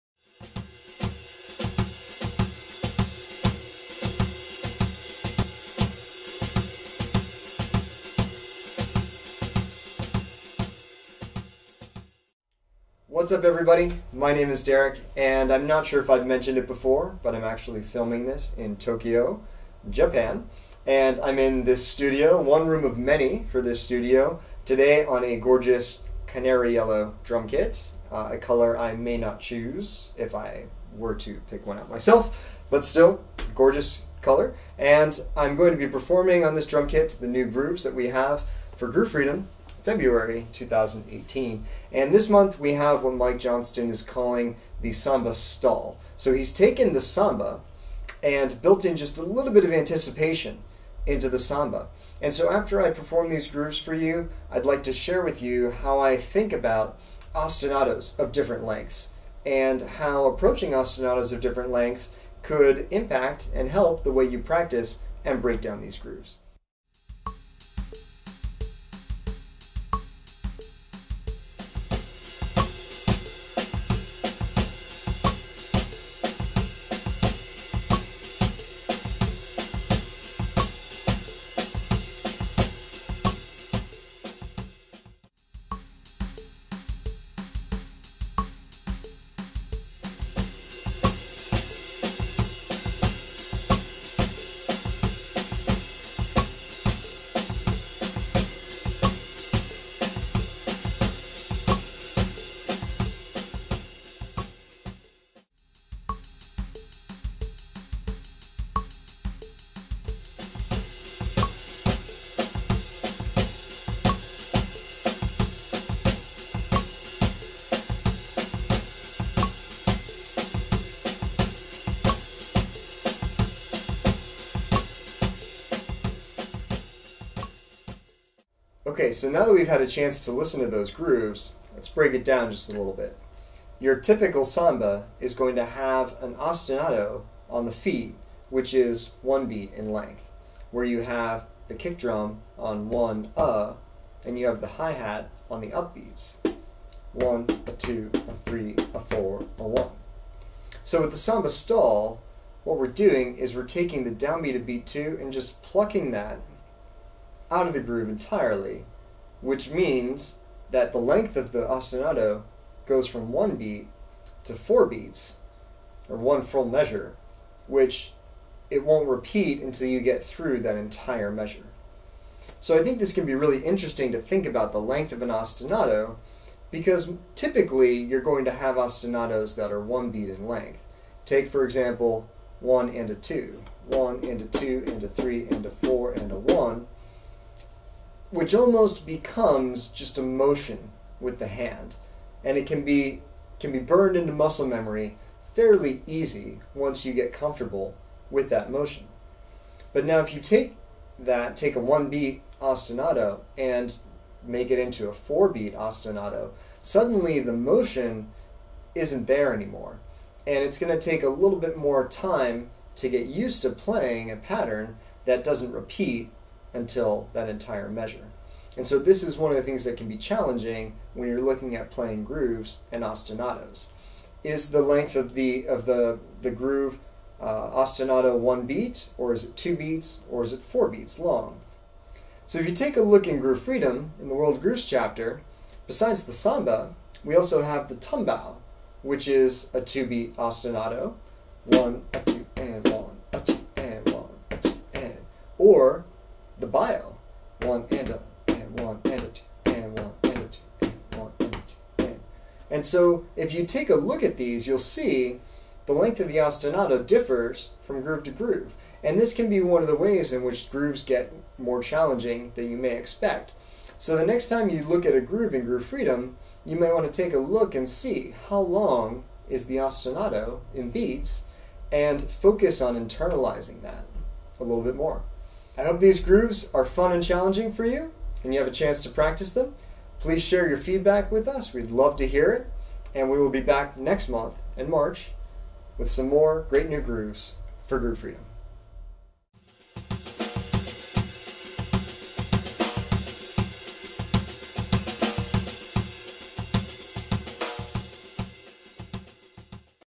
February 2018 New Grooves for Groove Freedom
February 2018 new grooves for Groove Freedom are now available for download!

Make sure you've updated to the latest version of GF (version 1.4 as of Feb 2018) and simply tap on the Settings tab and then tap on "Check for New Content" to download the following three new Snare Chapter grooves:

Chapter 3: 8th Notes on Ride / Samba Stall
Chapter 3: Syncopated Ride #1 / Samba Stall
Chapter 3: Syncopated Ride #2 / Samba Stall
Chapter 3: Syncopated Ride #3 / Samba Stall

Mike J creates some anticipation within the samba groove by dropping the note on beat #2 giving this variation on the traditional samba groove a unique feel. This is a great way to challenge your playing by turning the 1-beat long samba ostinato into a 4-beat long ostinato since it doesn't repeat until the entire measure comes back around.

These grooves are available for download immediately in Groove Freedom so please check them out, let us know what you think, and share your progress with us!

Best, Derek